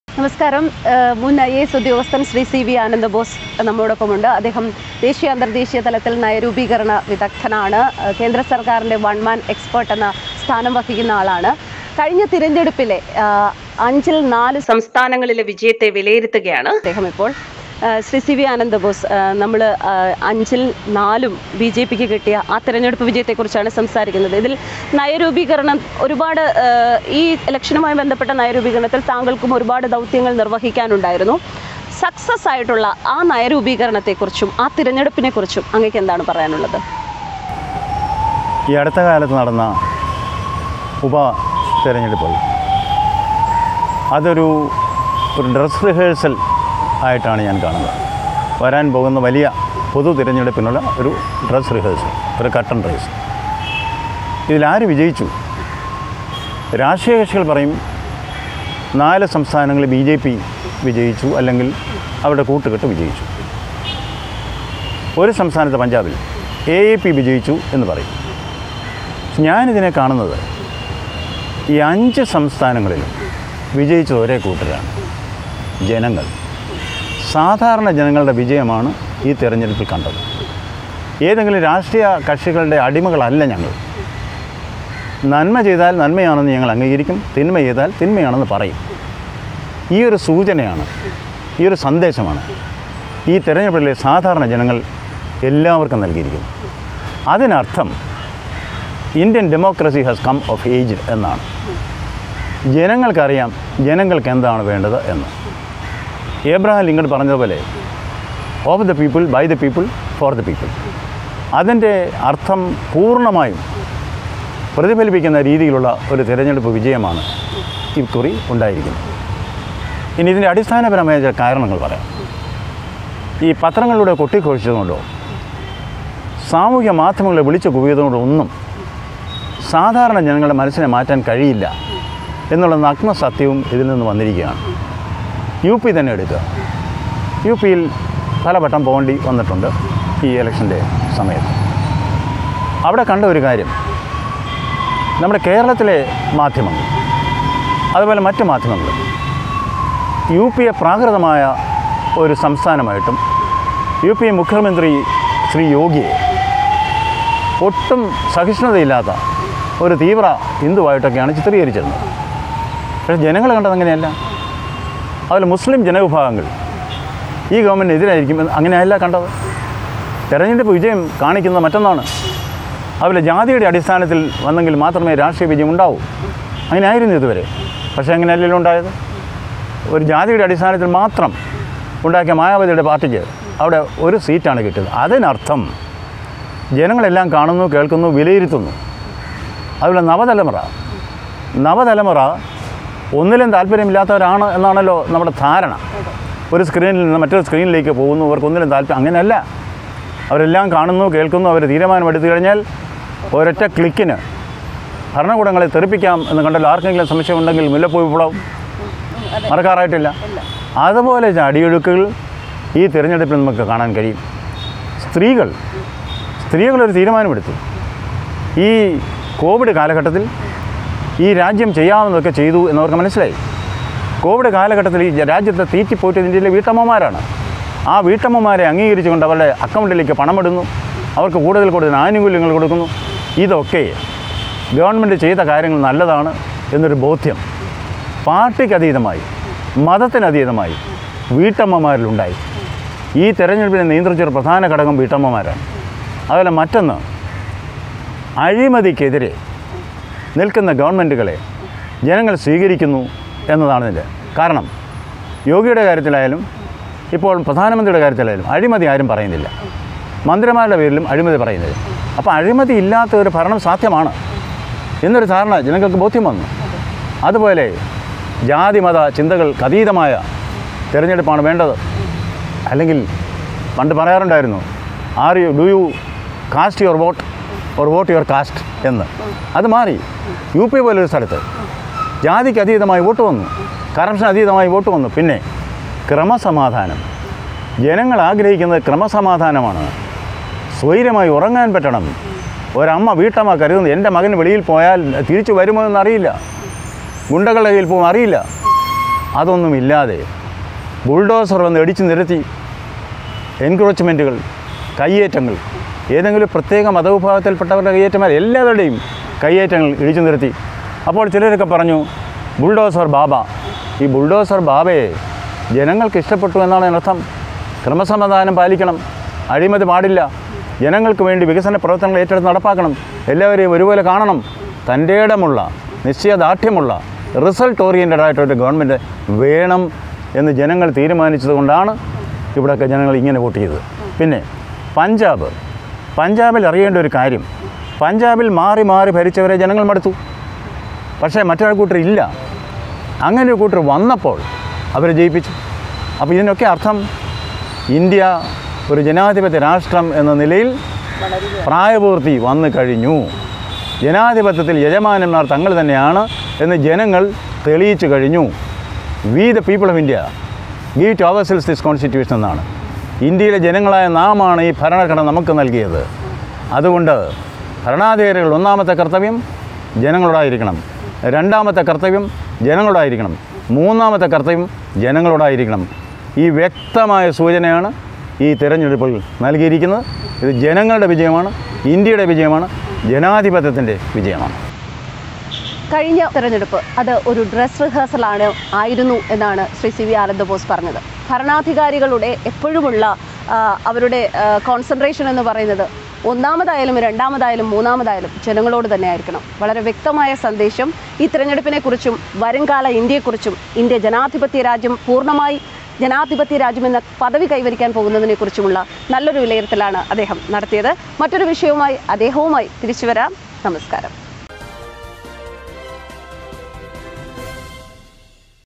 ബിജെപിയുടെ അമ്പരപ്പിക്കുന്ന ജയങ്ങൾക്ക് പിന്നിൽ dr c v ananda bose
5ൽ 4 സംസ്ഥാനങ്ങളും തൂത്തുവാരിയ ബിജെപിയുടെ വിജയത്തിനു പിന്നിൽ, ദേശീയ നയ രൂപീകരണ വിദഗ്ദൻ  dr c v ananda bose, ഡോ സി വി ആനന്ദബോസ്